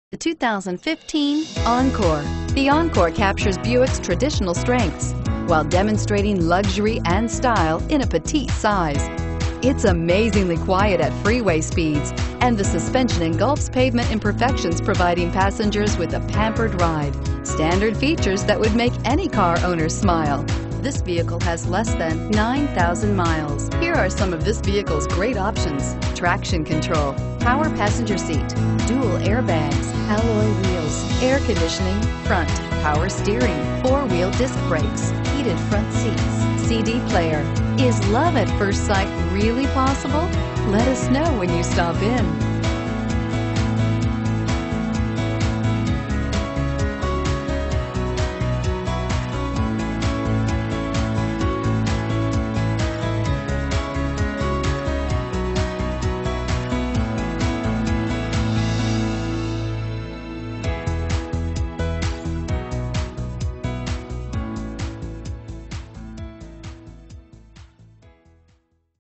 2015 Buick Encore Casper WY T7254A
2015 Buick Encore Premium

3333 CY Ave.

Don't miss this 2015 Buick Encore. It's equipped with Automatic transmission and features a White Pearl Tricoat exterior. With 8232 miles, You'll want to take this car home. Make a great choice today.